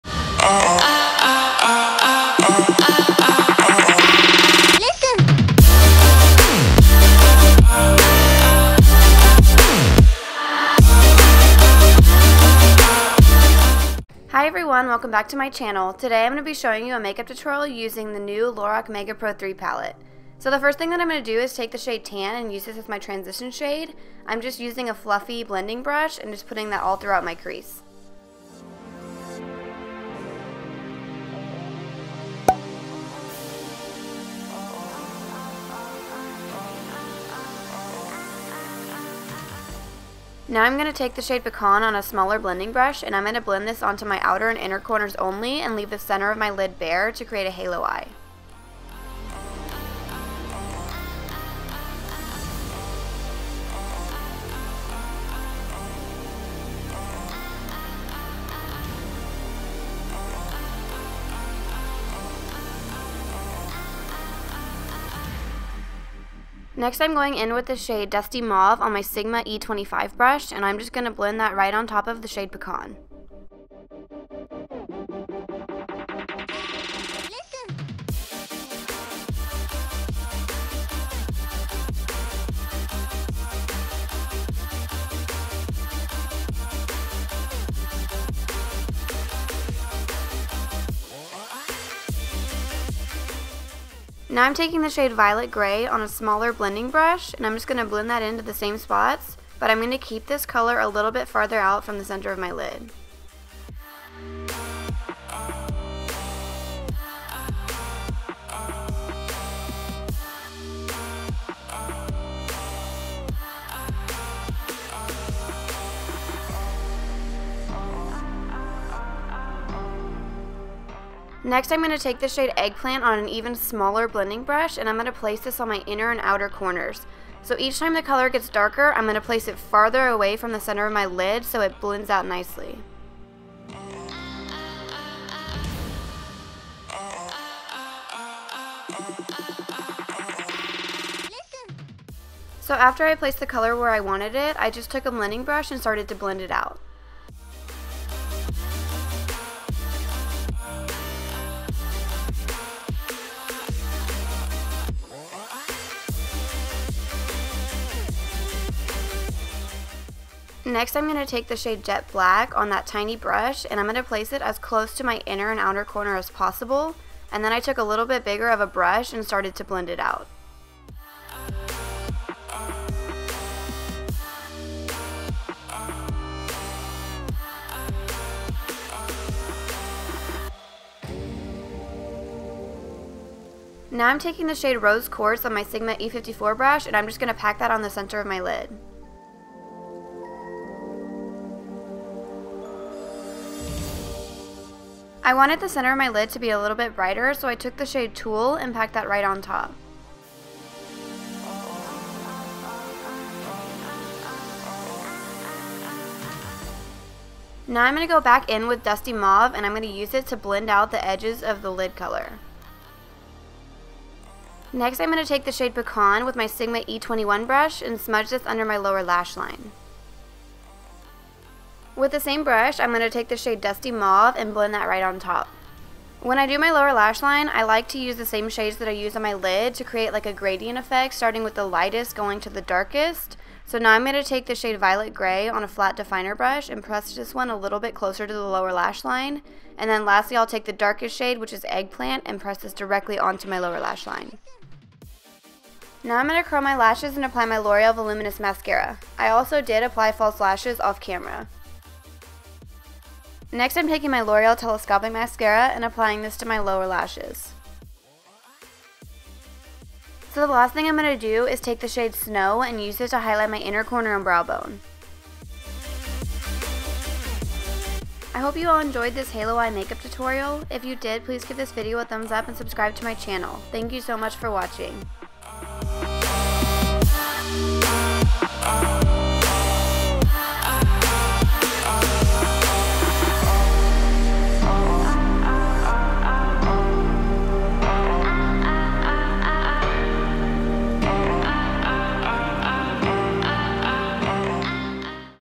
Halo Eye | Lorac Mega Pro 3 Palette | Makeup Tutorial
Hi everyone! Here is a halo eye makeup tutorial using the new Lorac Mega Pro 3 Palette! I hope you all enjoy!

PRODUCTS:
Lorac Mega Pro 3 Palette
(Limited Edition)
Loreal Voluminous Mascara - Carbon Black
Loreal Telescopic Mascara - Carbon Black
Ardell Studio Effects - 110
House of Lashes Eyelash Adhesive

BRUSHES:
IT Brushes for Ulta - Airbrush Blending Crease #105
Beau Gâchis Paris - Smudge Brush

EQUIPMENT:
Canon EOS Rebel SL1 DSLR Camera
Canon EF 50mm f/1.8 STM Standard Lens
Soft Box Lighting Kit
Backdrop Stand
Sequin Backdrops
Paper Backdrops